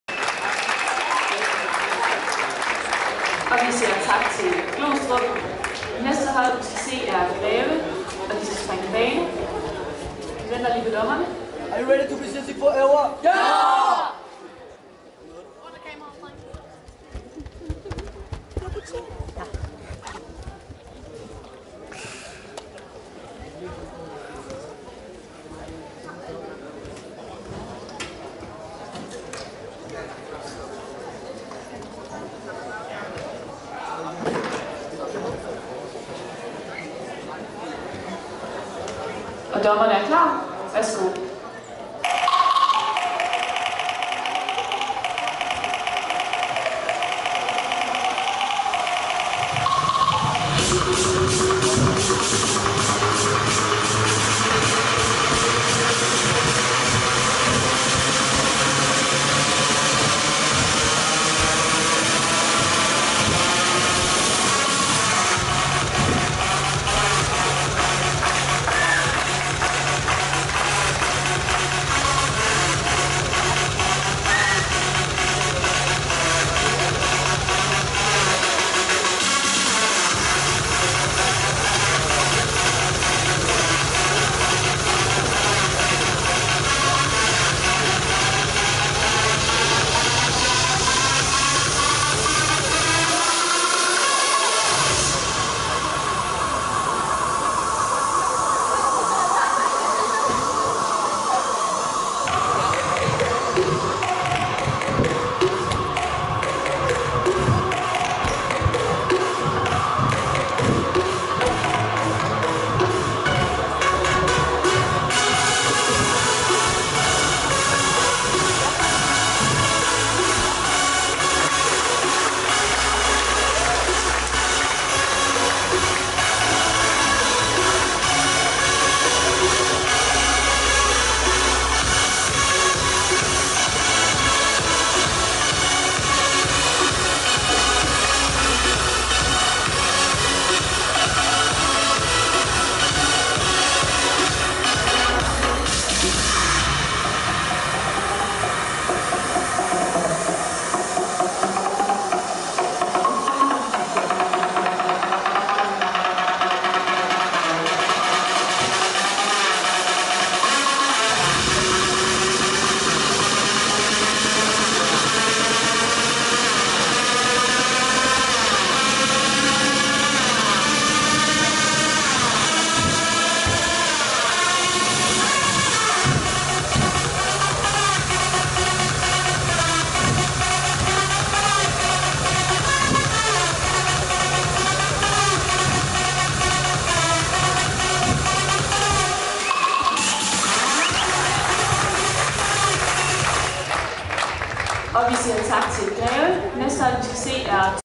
Junior DM Kvalifikation 2014 - Bane
Greve TeamGym junior drenge på bane ved Junior DM kvalifikation i Århus den 27. april 2014